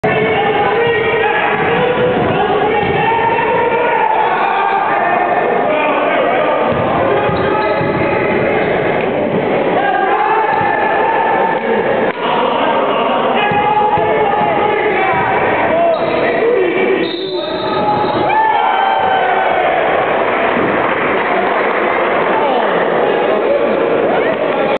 miaa b basketball playoffs: Boys' Latin vs. Curley, 2-17-2010
Wells Stanwick gets the three and one vs. Curley